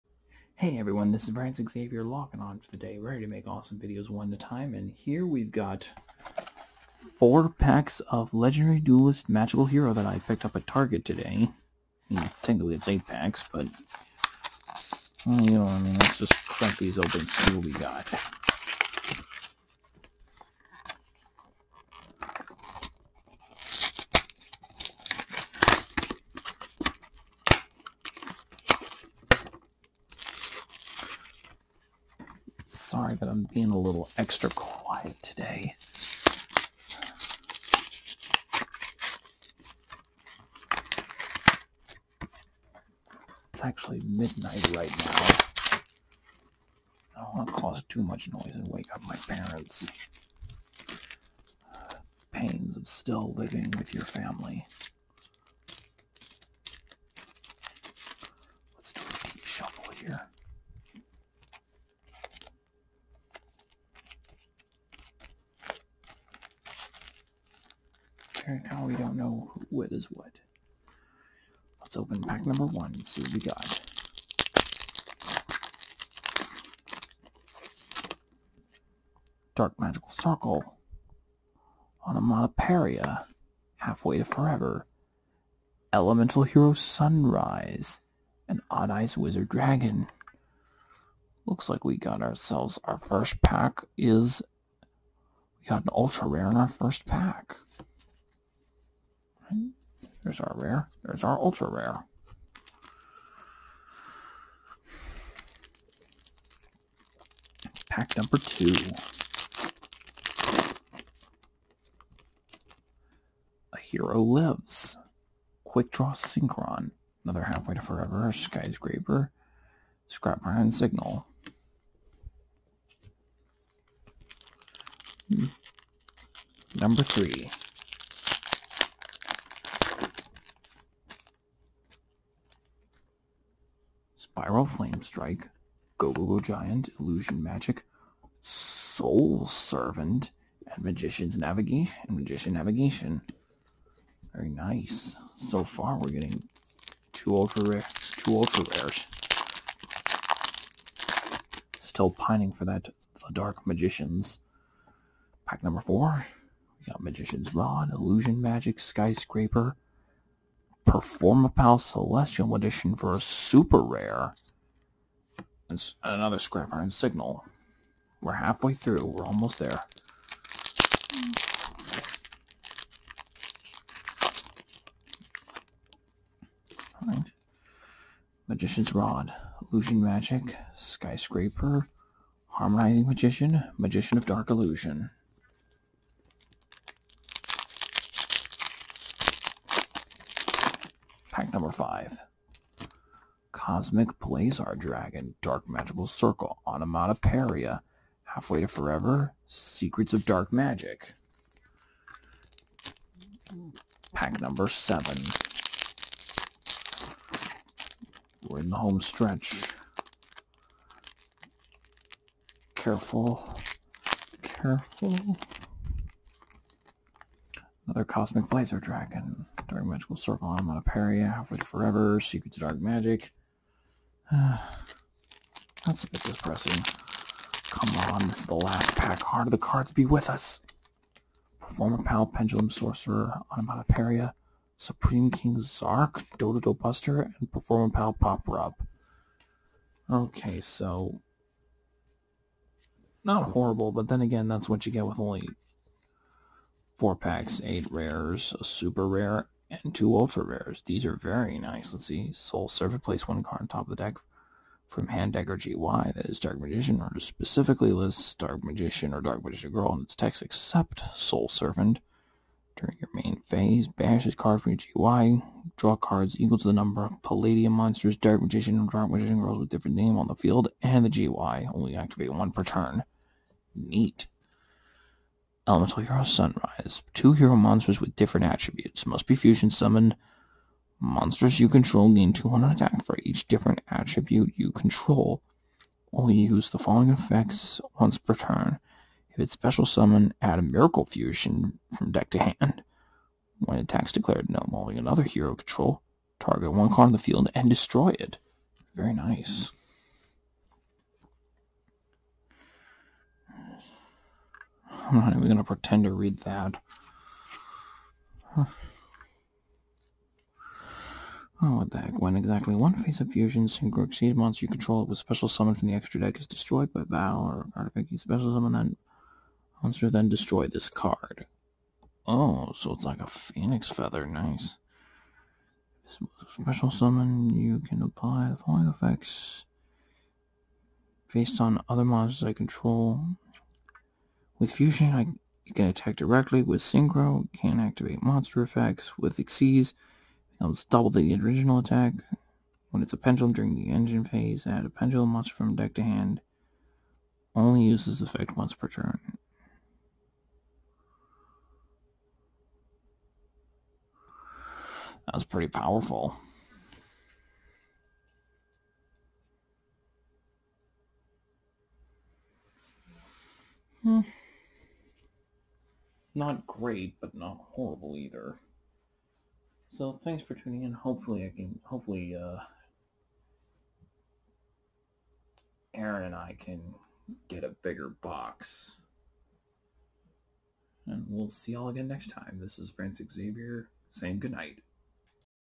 Francis Unpacks: Legendary Duelists: Magical Hero Mini-Unpacking
Here we go with another unpacking of 8 packs of Legendary Duelists: Magical Hero, and some pretty good pulls happened! Nothing HUGE, but nothing to scoff at! More to come soon!

-Francis Xavyer logging off!